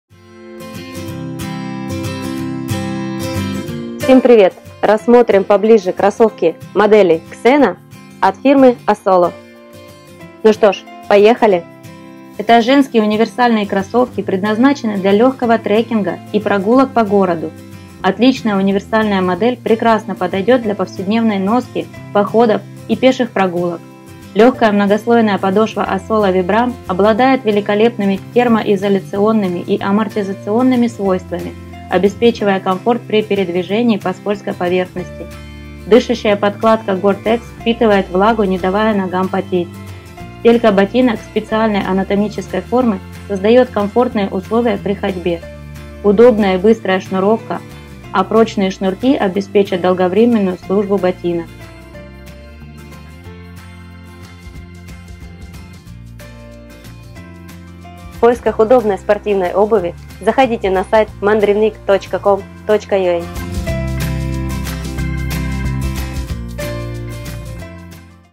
Кроссовки Asolo 6205 Xena ML
Кроссовки Asolo 6205 Xena ML - уже сняты с производства, но в нашем магазине Вы по прежнему можете найти большой ассортимент актуальных моделей

Кроссовки Asolo 6205 Xena ML - отличная универсальная модель прекрасно подойдет для повседневной носки, походов и пеших прогулок. Легкая многослойная подошва Asolo Vibram обладает великолепными термоизоляционными и амортизационными свойствами, обеспечивая комфорт при передвижении по скользкой поверхности. Дышащая подкладка Gore-Tex впитывает влагу, не давая ногам потеть. Стелька ботинок специальной анатомической формы создает комфортные условия при ходьбе. Удобная и быстрая шнуровка ботинок, прочные шнурки обеспечат долговременную службу ботинок.